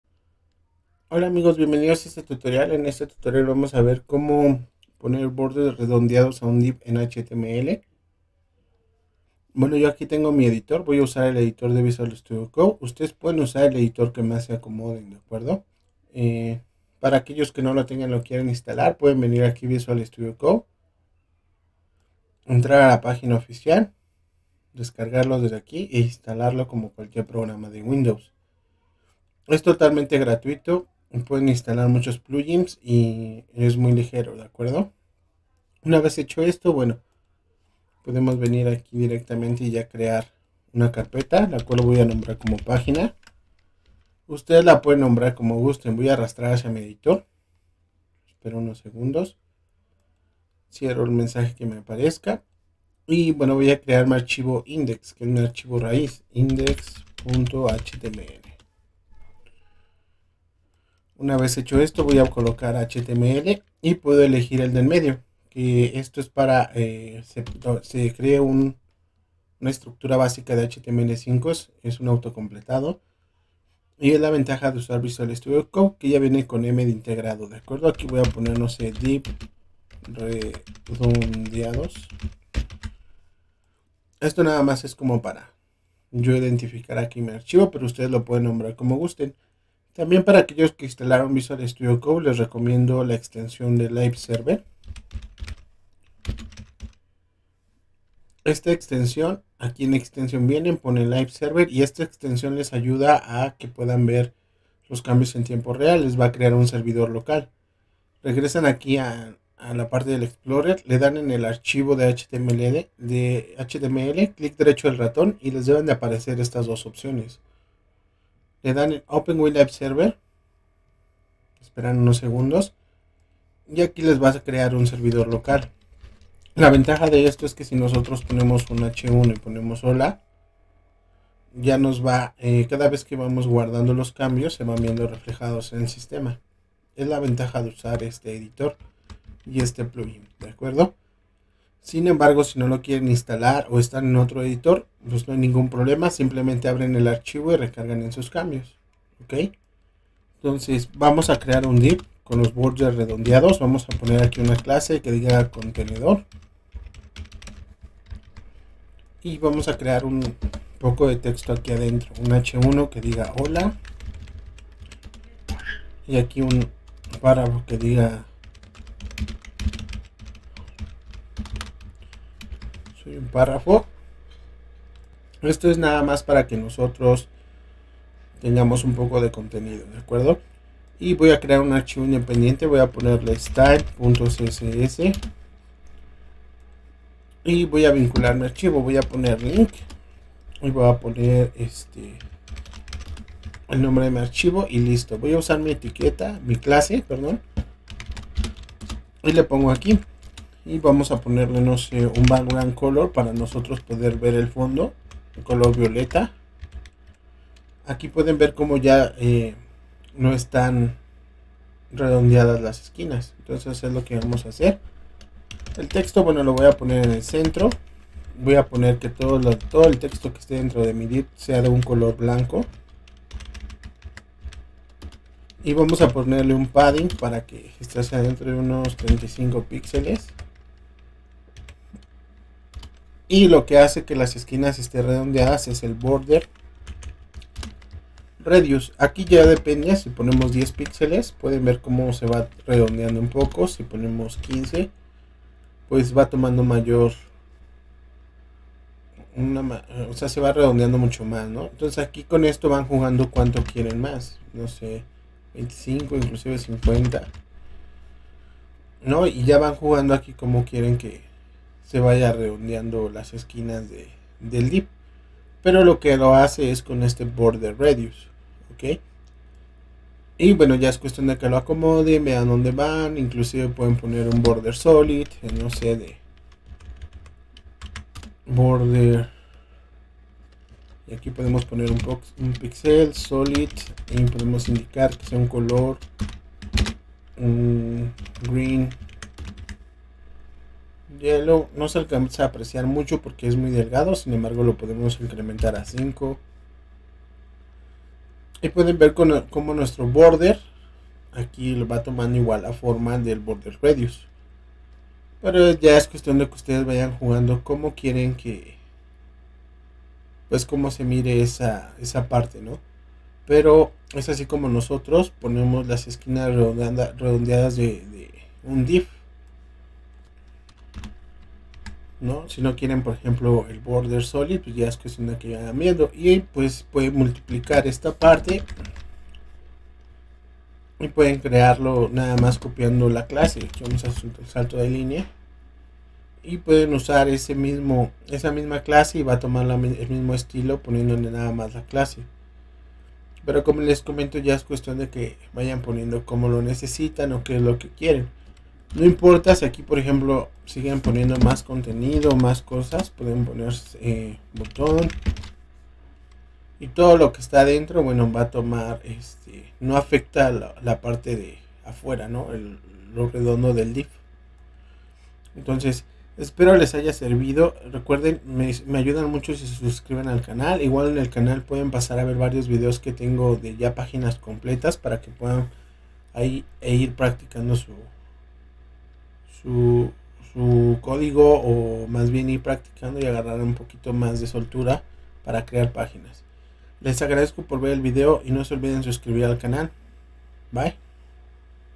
como poner BORDES redondeados a un DIV en HTML 🍇
En este tutorial veremos como poner bordes redondeados a un div en html